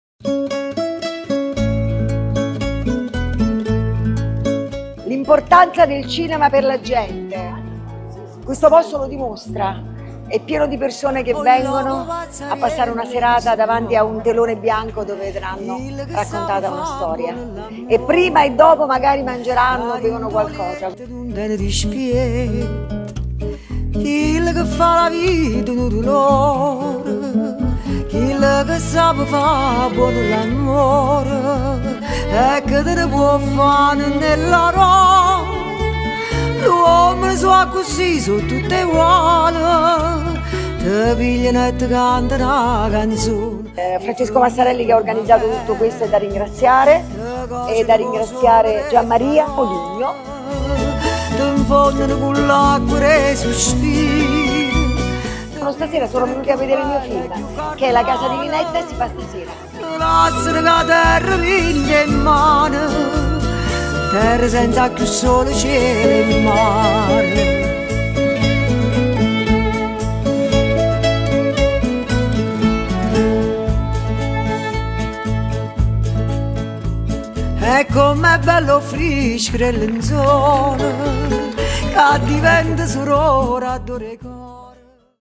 “Capua Film Fest”,tripudio per Lina Sastri. I Giardini dello Sperone presi d’assalto
“Capua Film Fest”, tripudio per Lina Sastri. I Giardini dello Sperone presi d’assalto. Oggi nuovo appuntamento